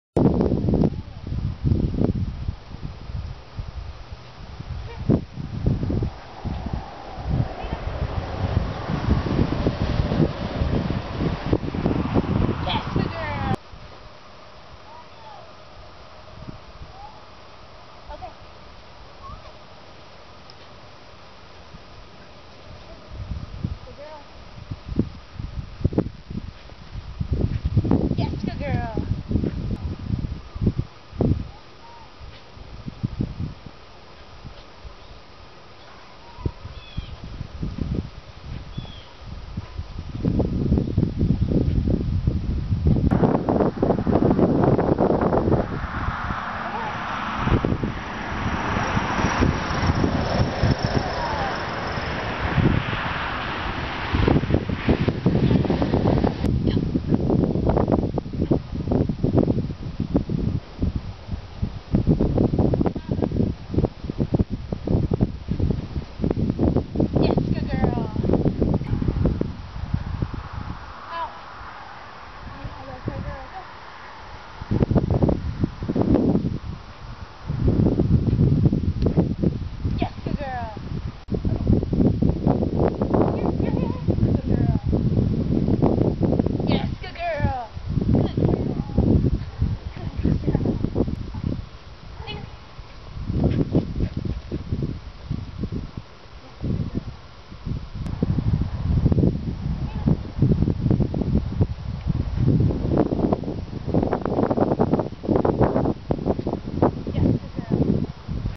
StarBox Drills
Athena working on Star Box Drills for the first time. :)